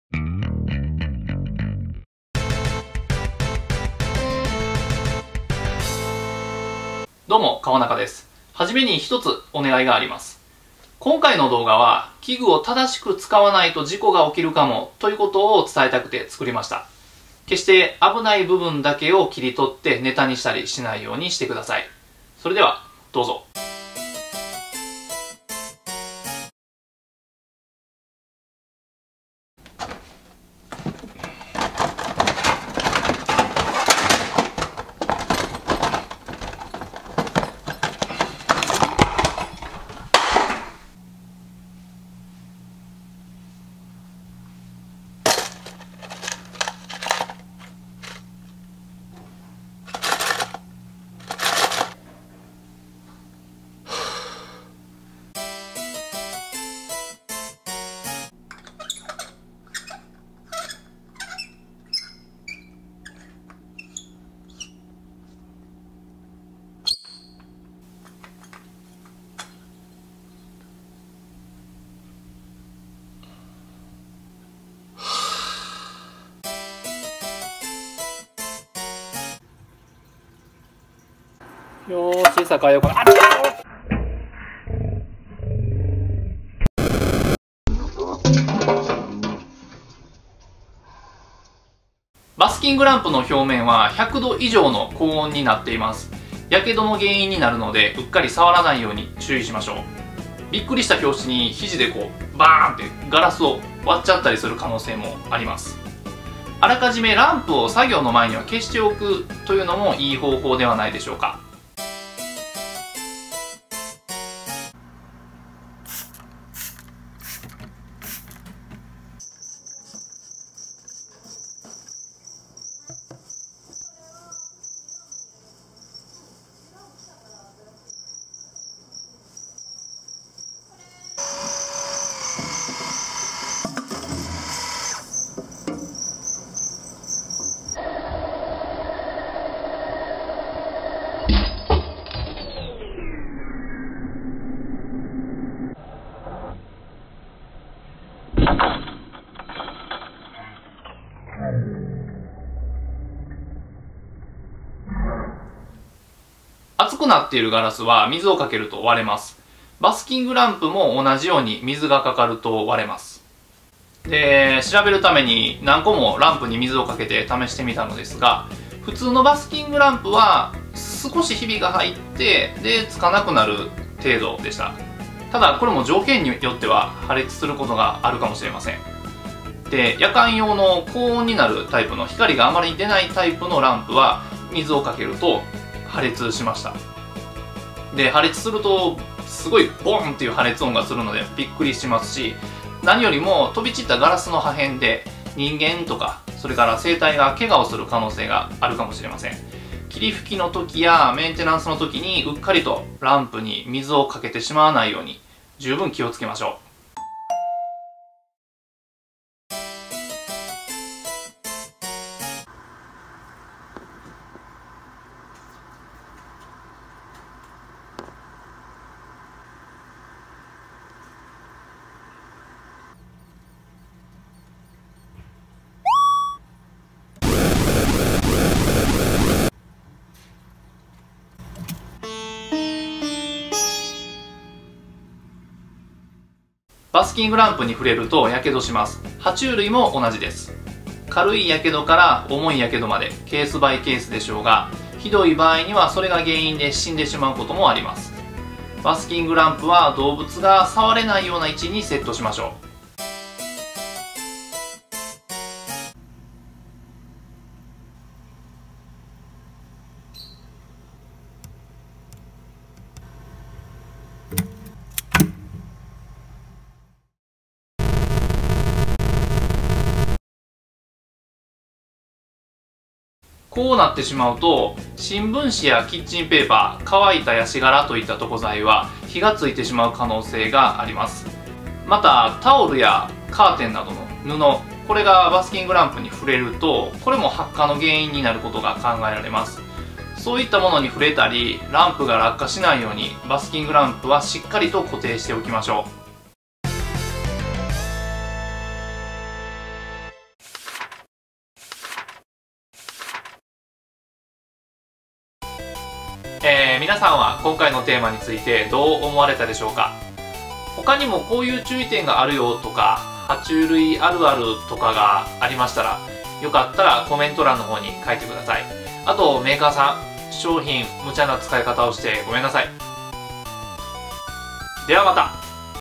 【爬虫類】バスキングランプを使うときの注意点トップ５｜Top5 things not to do with reptile lamps! (with English subtitles)
注：新しい商品にはさらなる改良が施されている場合があります。

「落とす」と「締めすぎる」には、球切れしたバスキングランプを使っています。

動画で取り上げた内容には事故やケガを招く可能性があります。真似しないでください。

アマゾンほしい物リスト。ご支援いただけると助かります！